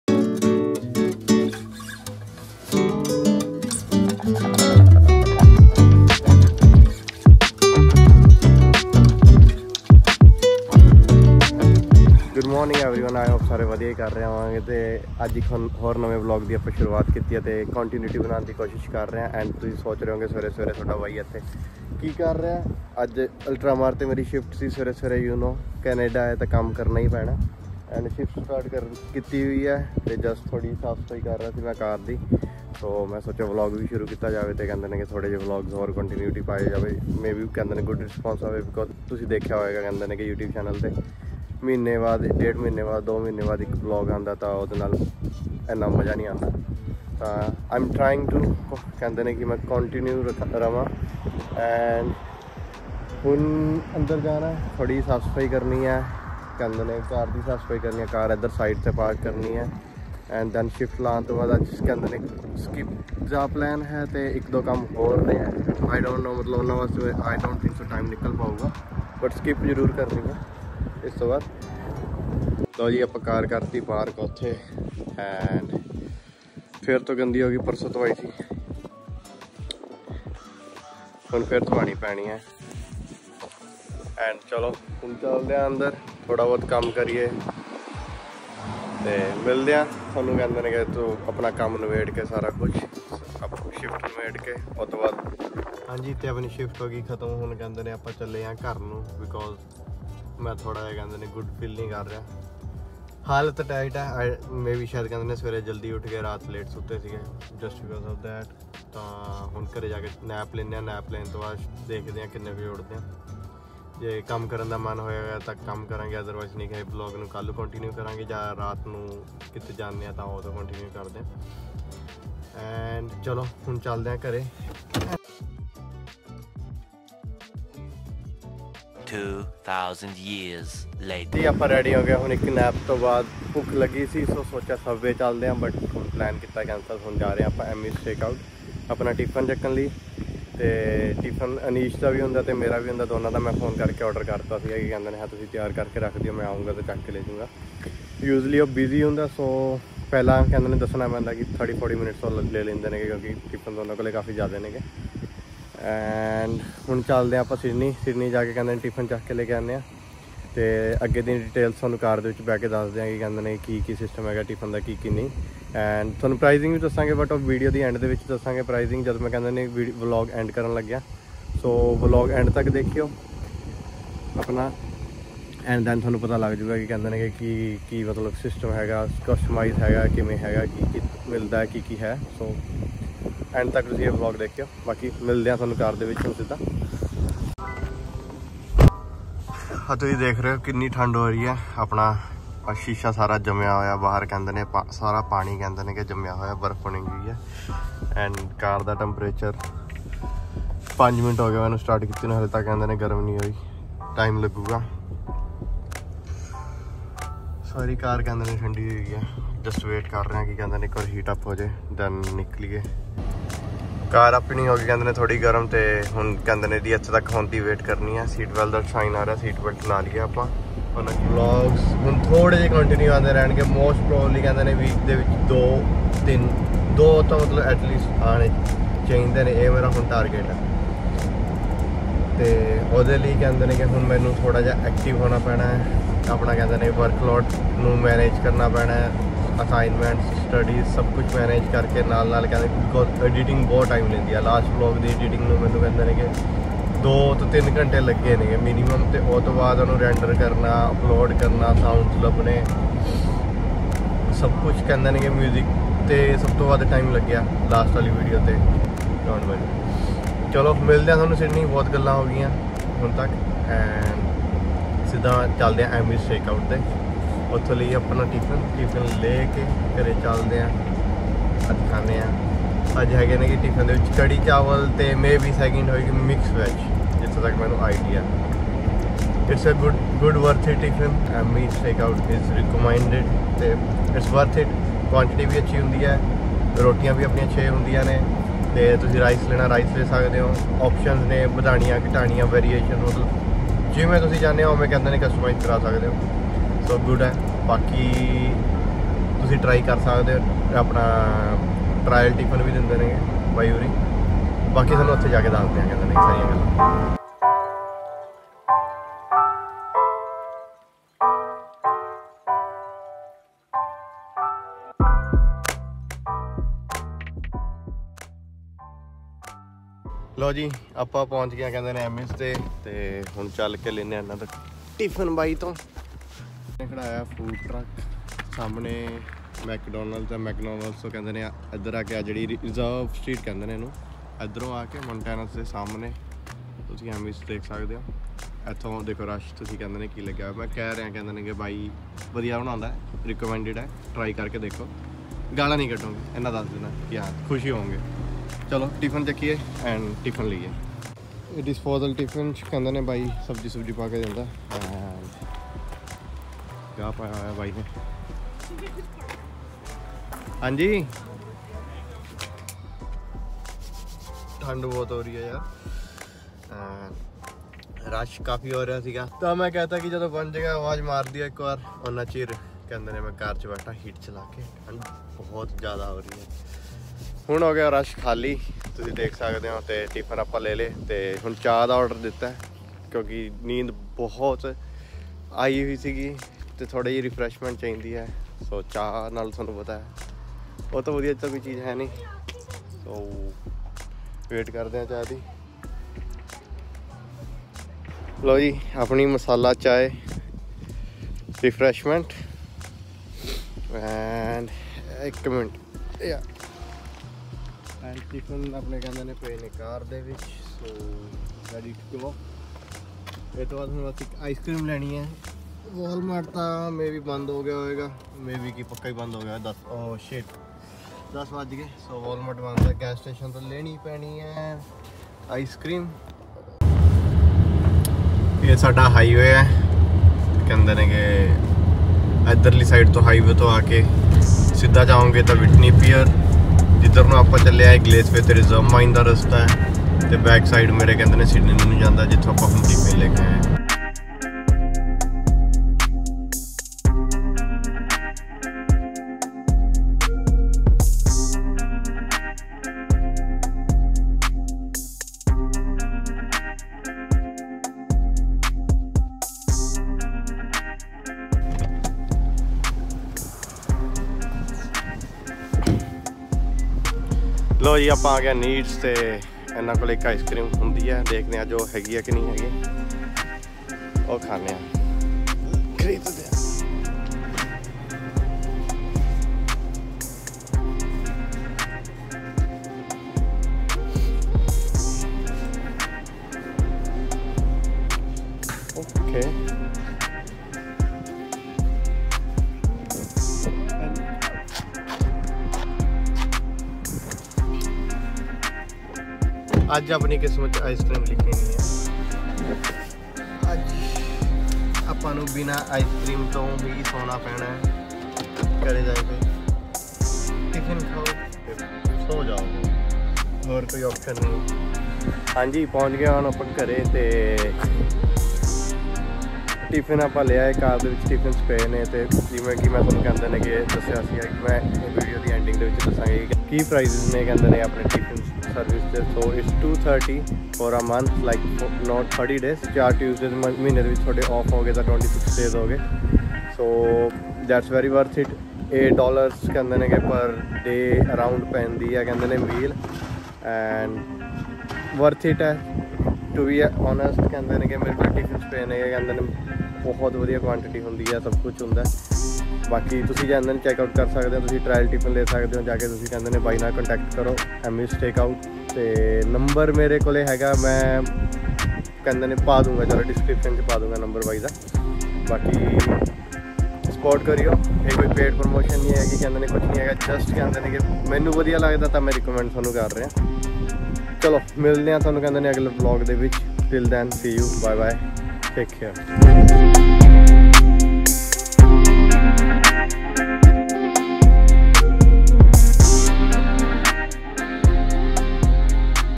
Tiffin Service in Sydney, Nova Scotia (CANADA) | Price | punjabi tiffin service Sydney
Tiffen service in Canada 🇨🇦 || price of Tiffen in Canada || benefits || for students.

Location-  Ammy's Takeout (30 Reeves St, Sydney, NS B1P 3C5, Canada)

Trying my best to upload as frequently as possible